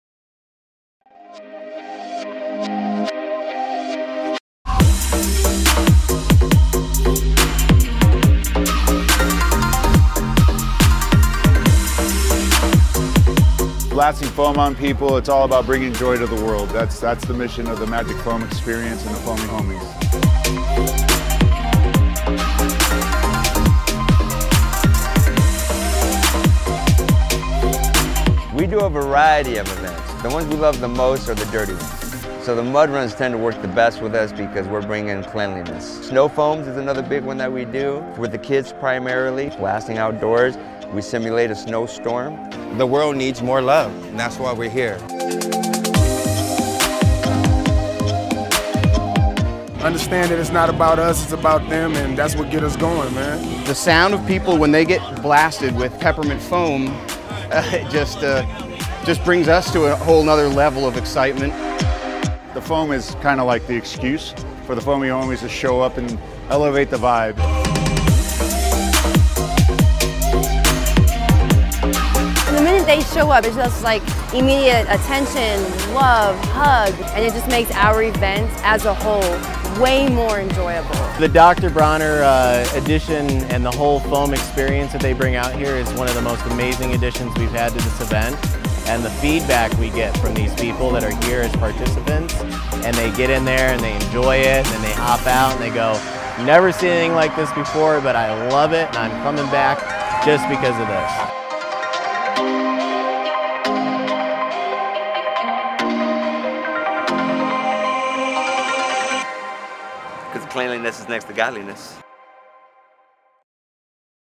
Dr. Bronner's Magic Foam Experience!
Dr. Bronner’s Magic Foam Experience was created to honor the late Jim Bronner (son of Emanuel Bronner), who invented an industry-standard fire-fighting foam concentrate in the 1980s. He later modified his foam to make artifical snow for Hollywood sets, and he even created a foaming unit for blasting snow-like foam in the backyard when his children were growing up.

Dr. Bronner’s All-One Magic Foam Experience is a novel, ecological and exhilarating foam “shower” that covers you in foam and leaves you feeling refreshed and tingly clean. Made simply from Dr. Bronner’s soap, water, and compressed air, our foam is safe and biodegradable. People of all ages love to play in the foam, and spaces can be quickly transformed into a magical wonderland.

We bring the Magic Foam Experience — often accompanied by our Magic Fire Truck — to public schools, non-profit children’s centers, parades, mud-runs, beach events, community events, and more.